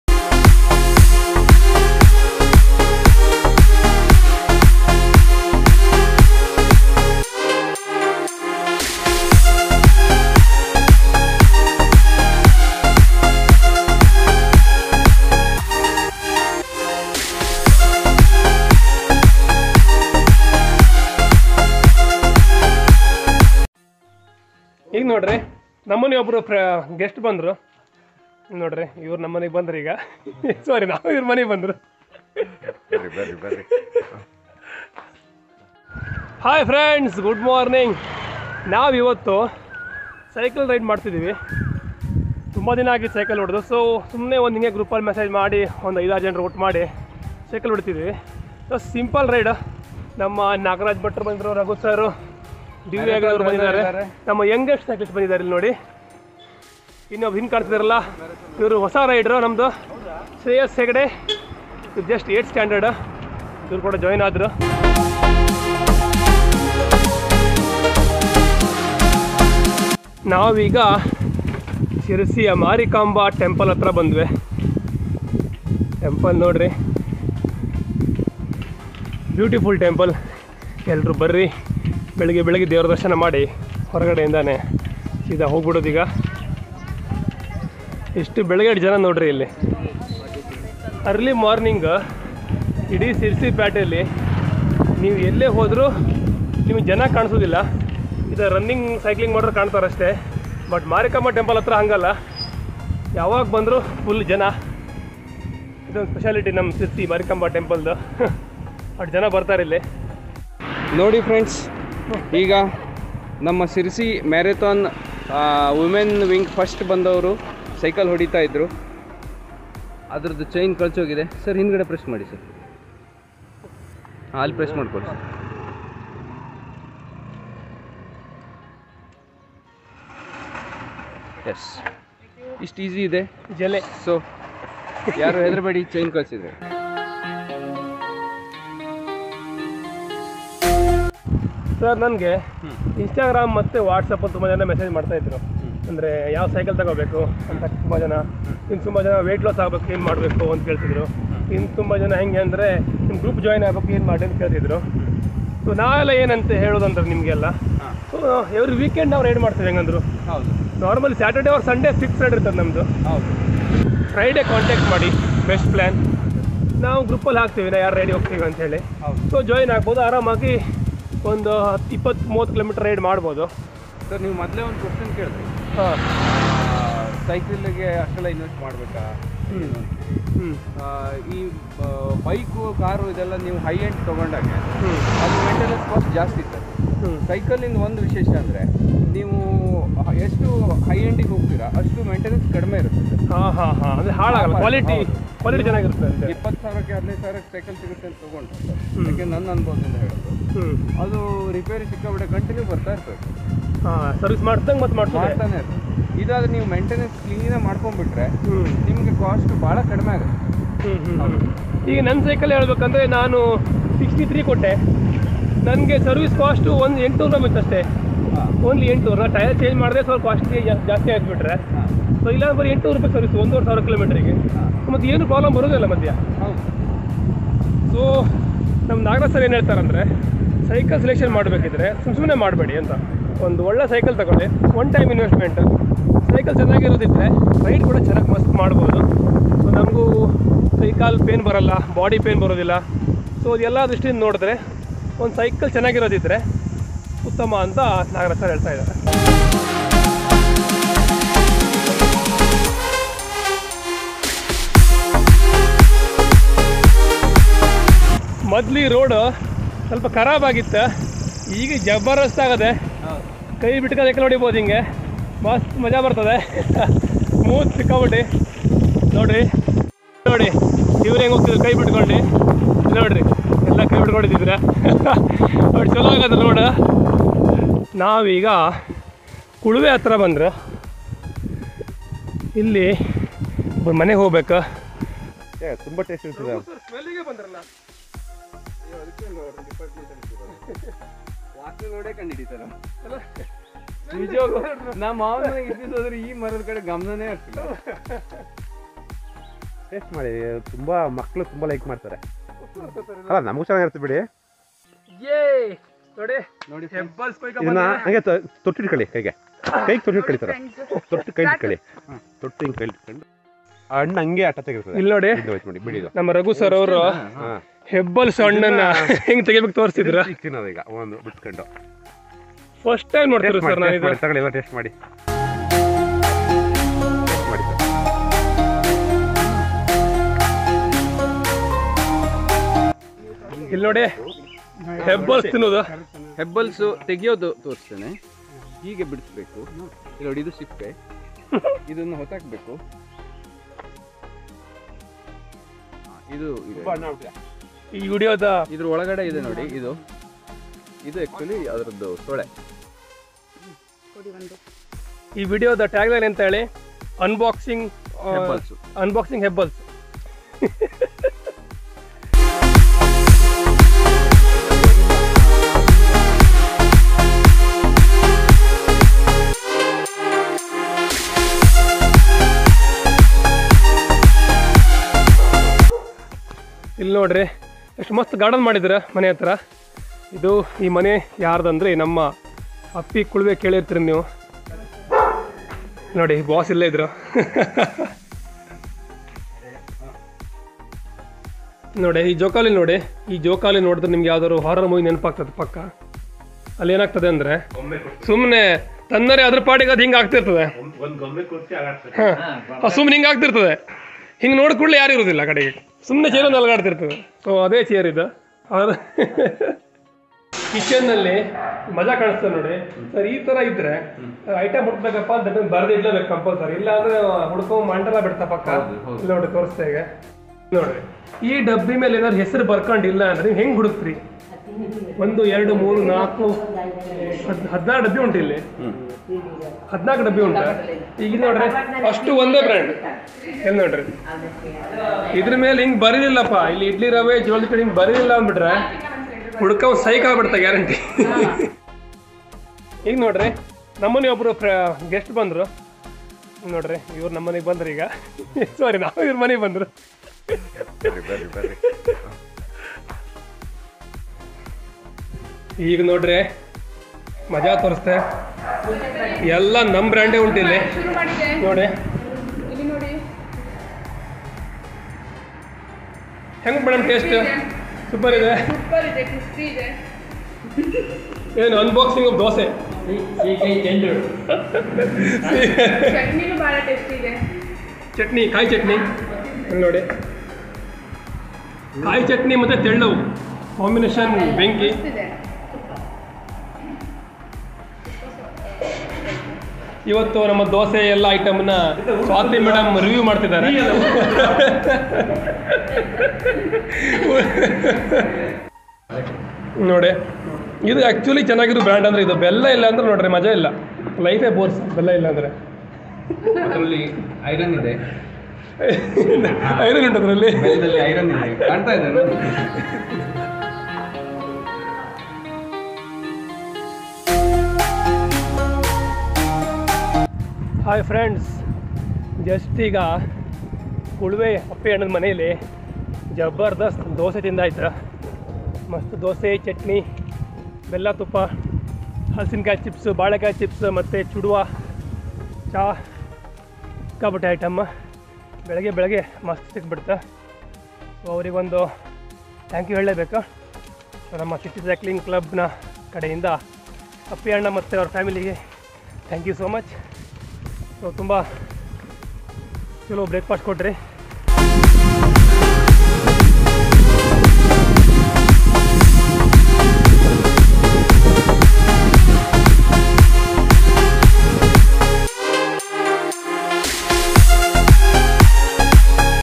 Morning Vibes | Breakfast ride to Kulve | Nature ride | jungle cycling
Morning Cycle ride to Kulve,
Breakfast ride to kulve
Sirsi to kulve cycle ride